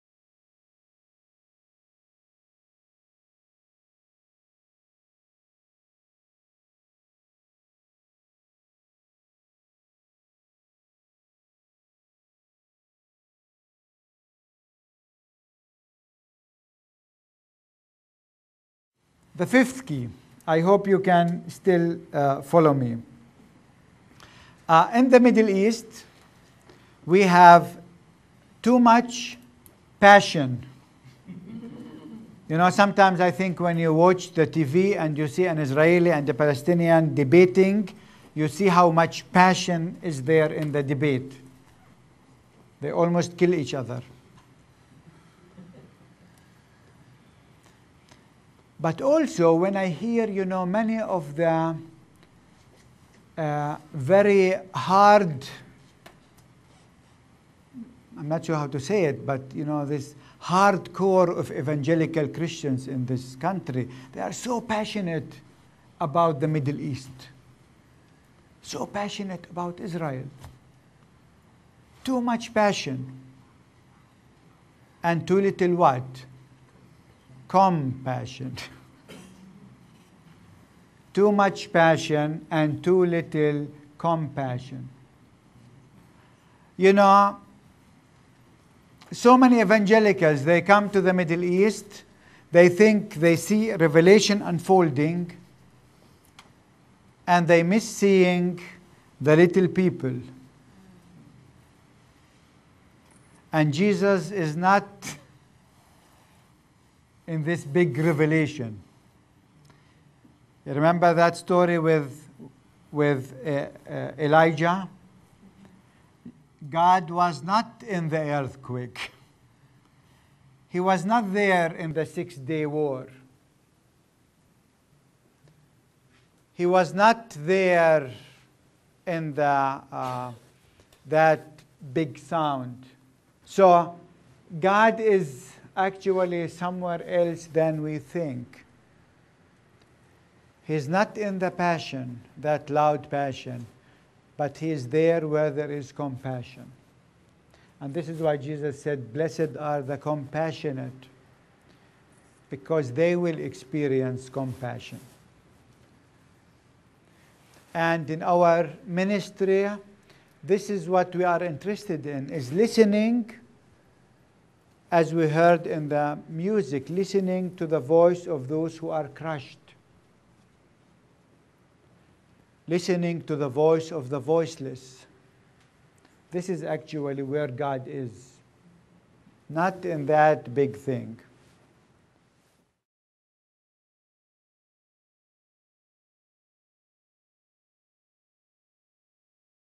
Part 5 Too much passion and too little compassion
Through the eyes of a Palestinian Christian: 7 keys to understanding the Israeli -- Palestinian conflict
Rev. Dr. Mitri Raheb
Pastor, Christmas Lutheran Church
Bethlehem, West Bank
Founder, Bright Stars of Bethlehem NFP